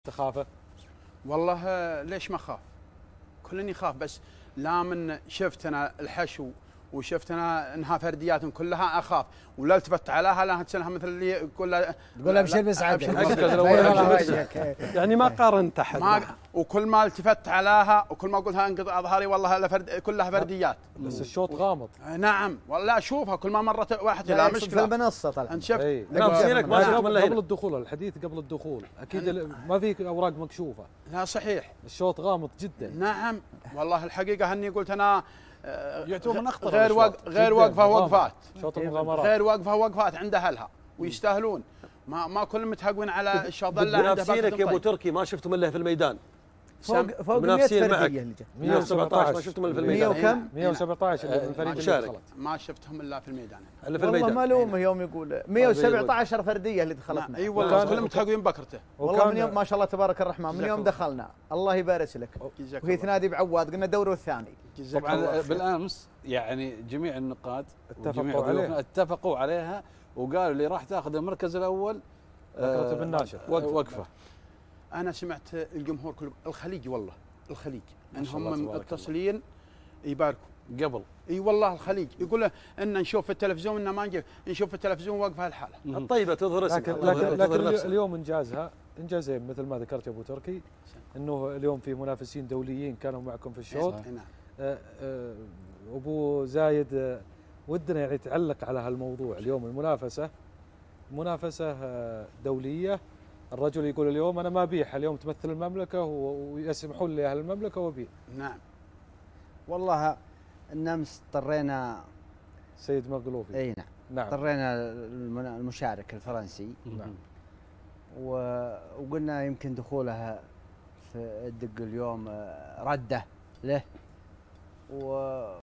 مجلس_الصياهد | عبدالله ناشر الدوسري، وقفة وقفات في عيون أهلها
عبدالله ناشر الدوسري:
في الشوط اليوم غير "وقفة" وقفات في عيون أهلها.